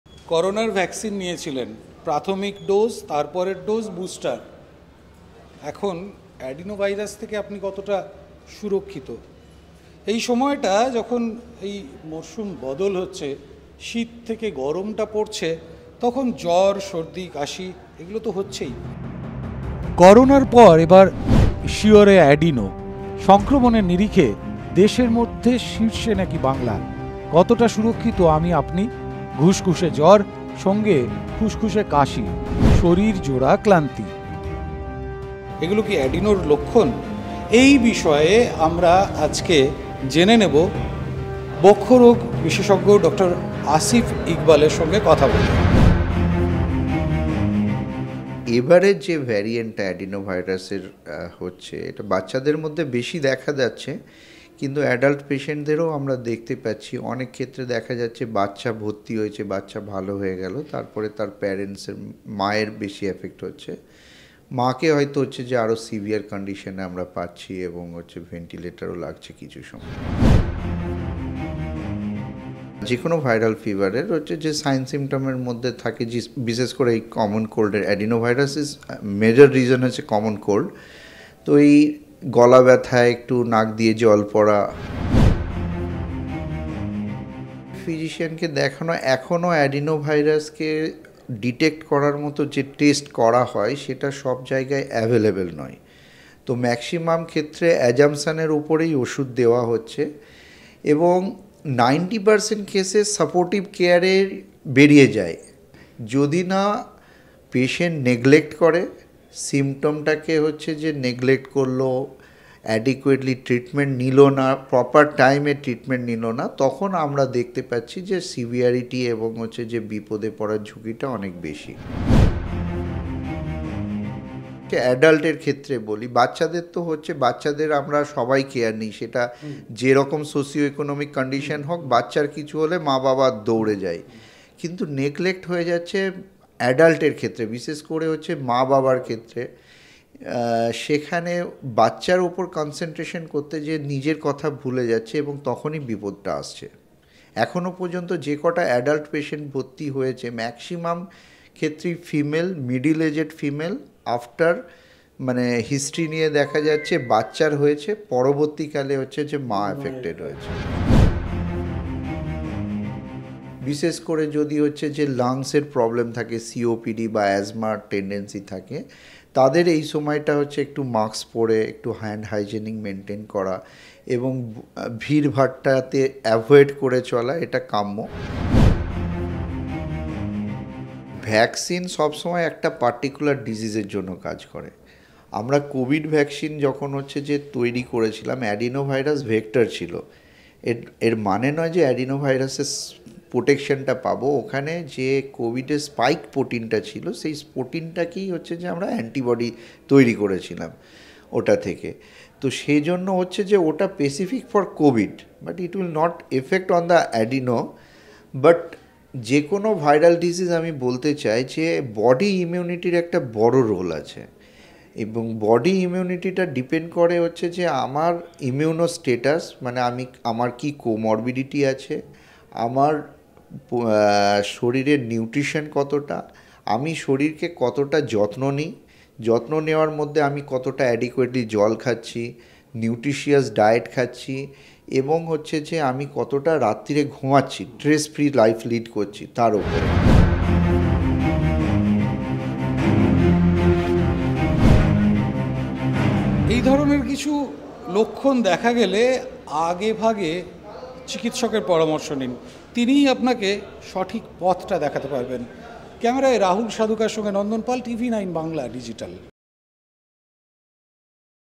Adenovirus News: কী করে বুঝবেন আপনি অ্যাডিনোয় আক্রান্ত?
করোনার পর এবার শিয়রে অ্যাডিনো। সংক্রমণের নিরিখে দেশের মধ্যে শীর্ষে নাকি বাংলা। কতটা সুরক্ষিত আমি আপনি? ঘুসঘুসে জ্বর। খুসখুসে কাশি। শরীর জোড়া ক্লান্তি।